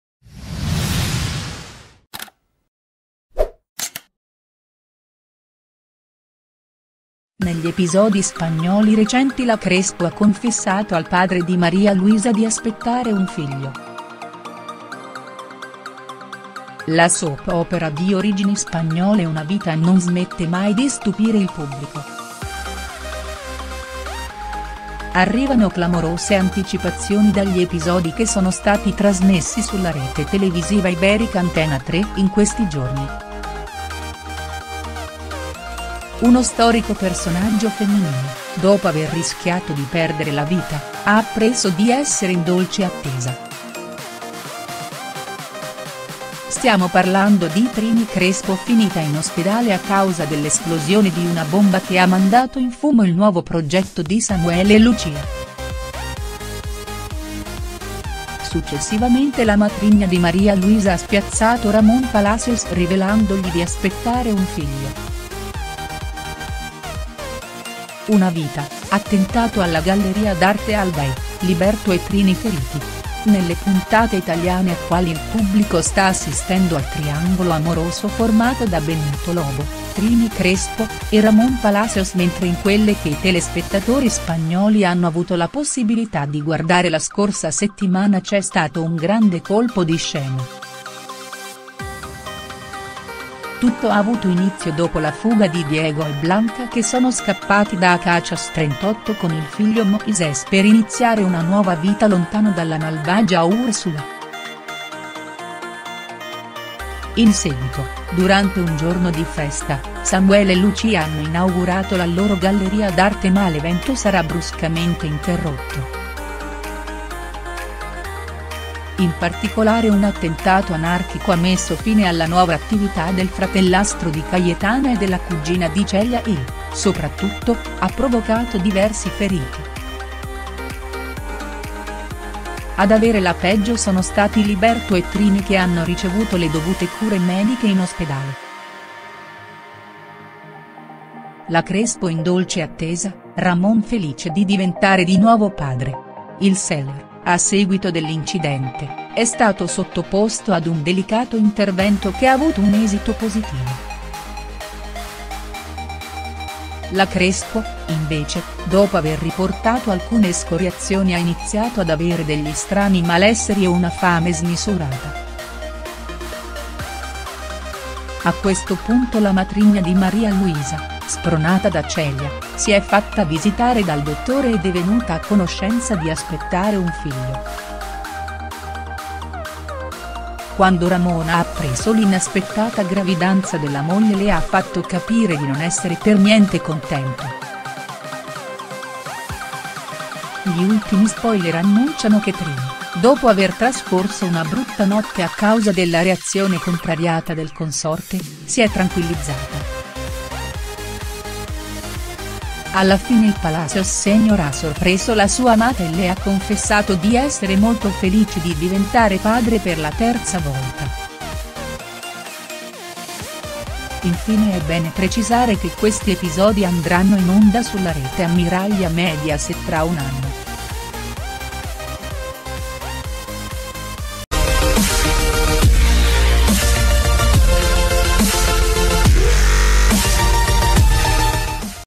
Anticipazioni Una Vita: Trini annuncia a Ramon di essere in dolce attesa
Negli episodi spagnoli recenti la Crespo ha confessato al padre di Maria Luisa di aspettare un figlio. La soap opera di origini spagnole “Una Vita”  non smette mai di stupire il pubblico. Arrivano clamorose anticipazioni dagli episodi che sono stati trasmessi sulla rete televisiva iberica “Antena 3” in questi giorni. Uno storico personaggio femminile, dopo aver rischiato di perdere la vita, ha appreso di essere in dolce attesa. Stiamo parlando di Trini Crespo finita in ospedale a causa dell’esplosione di una bomba che ha mandato in fumo il nuovo progetto di Samuel e Lucia. Successivamente la matrigna di Maria Luisa ha spiazzato Ramon Palacios rivelandogli di aspettare un figlio. Una Vita: attentato alla galleria d’arte Alday, Liberto e Trini feriti. Nelle puntate italiane attuali il pubblico sta assistendo al triangolo amoroso formato da Benito Lobo, Trini Crespo, e Ramon Palacios mentre in quelle che i telespettatori spagnoli hanno avuto la possibilità di guardare la scorsa settimana c’è stato un grande colpo di scena. Tutto ha avuto inizio dopo la fuga di Diego e Blanca che sono scappati da Acacias 38 con il figlio Moises per iniziare una nuova vita lontano dalla malvagia Ursula. In seguito, durante un giorno di festa, Samuel e Lucia hanno inaugurato la loro galleria d’arte ma l'evento sarà bruscamente interrotto. In particolare un attentato anarchico ha messo fine alla nuova attività del fratellastro di Cayetana e della cugina di Celia e, soprattutto, ha provocato diversi feriti. Ad avere la peggio sono stati Liberto e Trini che hanno ricevuto le dovute cure mediche in ospedale. La Crespo in dolce attesa, Ramon felice di diventare di nuovo padre. Il Seler, a seguito dell’incidente, è stato sottoposto ad un delicato intervento che ha avuto un esito positivo. La Crespo, invece, dopo aver riportato alcune escoriazioni ha iniziato ad avere degli strani malesseri e una fame smisurata. A questo punto la matrigna di Maria Luisa, spronata da Celia, si è fatta visitare dal dottore ed è venuta a conoscenza di aspettare un figlio. Quando Ramon ha appreso l'inaspettata gravidanza della moglie le ha fatto capire di non essere per niente contento. Gli ultimi spoiler annunciano  che Trini, dopo aver trascorso una brutta notte a causa della reazione contrariata del consorte, si è tranquillizzata. Alla fine il Palacios senior ha sorpreso la sua amata e le ha confessato di essere molto felice di diventare padre per la terza volta. Infine è bene precisare che questi episodi andranno in onda sulla rete ammiraglia Mediaset tra un anno.